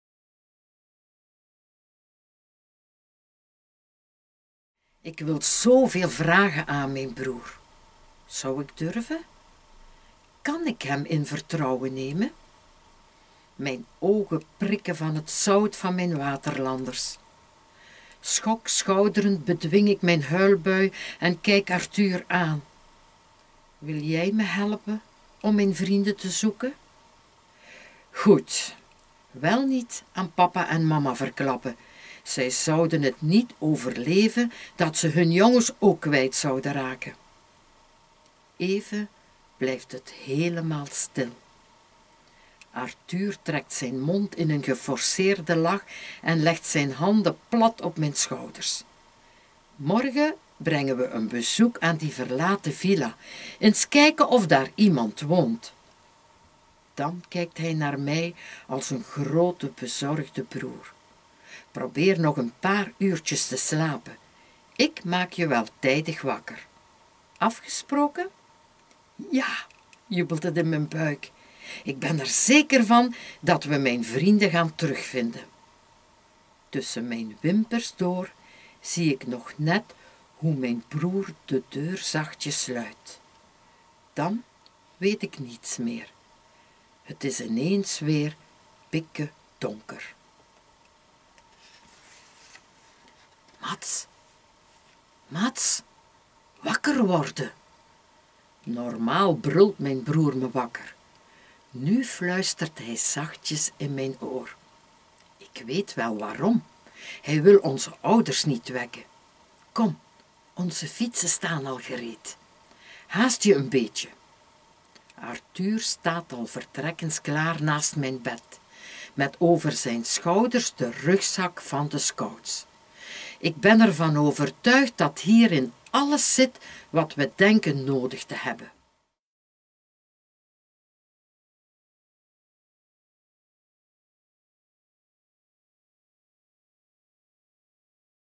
Marina Theunissen leest voor: De man met het groene masker
Mats weet één ding: hij zal zijn vrienden gaan zoeken, al moet hij  naar de maan reizen. Waar hij naartoe gaat, is de maan niet eens zo ver vandaan. Woetoemba? Is dit een plaats of een dorp? Dan toch een heel speciaal dorp, ergens dicht bij de Melkweg waar de Woetezen in een kolonie van ruimteschepen wonen, of zijn het ufo’s? Kwantum en Nanda, flitsarmbanden, verplaatsen in tijd en ruimte? Mats is ervan overtuigd dat hij zo dadelijk ontwaakt uit een heel vreemde droom. Verrassend genoeg is het geen droom, maar wat is het dan wel? Kan hij helemaal alleen zijn ontvoerde vrienden redden en Ximinas, de grote leider van de Woetezen, bevrijden? Komt er misschien hulp vanuit een onverwachte hoek? Meer nog, is hij moedig genoeg om de zware taak tot een goed einde te brengen?

Eén ding weet ik zeker: dit verhaal neemt je mee naar de wonderbare wereld tussen de sterren waar aliens wonen. Bangelijk, spannend, ongelooflijke sciencefiction!

Boeknummer 21978, voor jongeren vanaf 10 jaar die van een ruimte-avontuur houden!